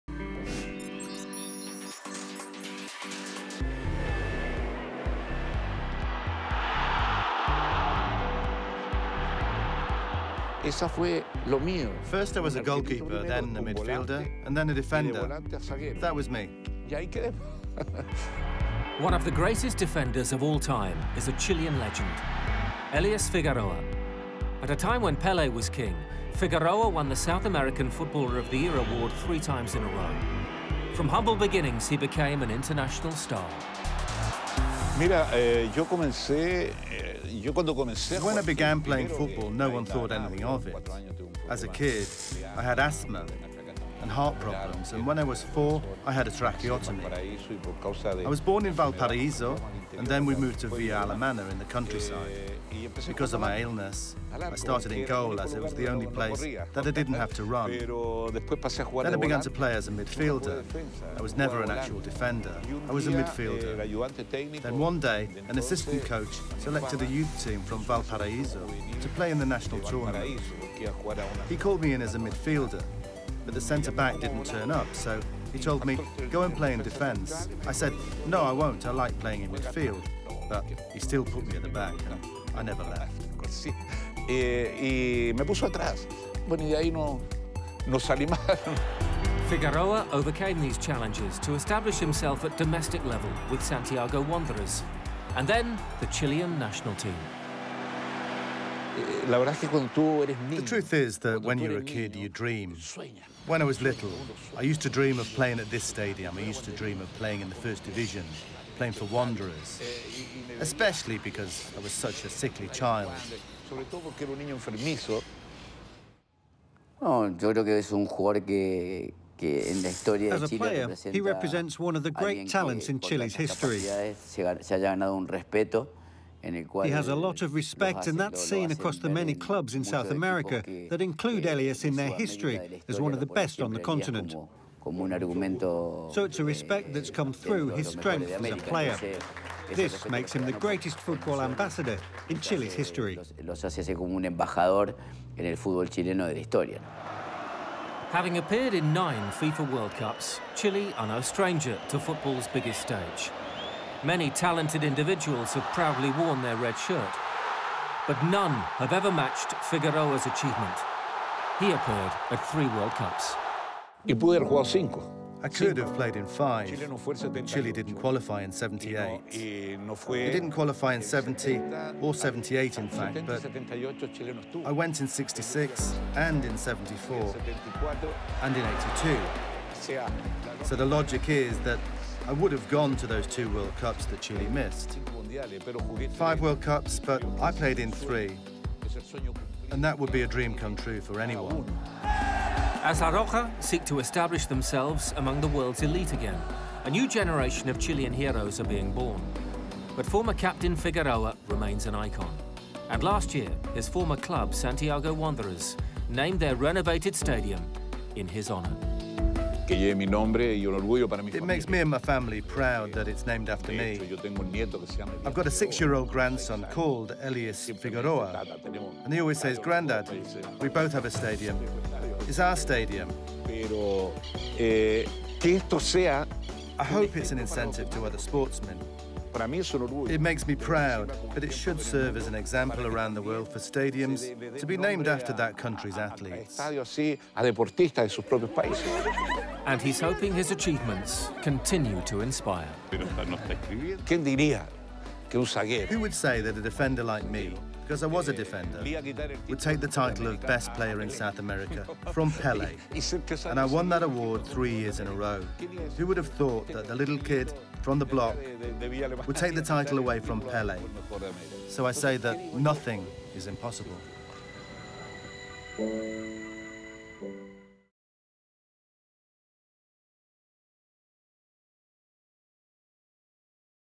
Chile legend Elias Figueroa - EXCLUSIVE
One of the greatest defenders of all time, the Chilean icon remembers to FIFA Football his three FIFA World Cups and many awards.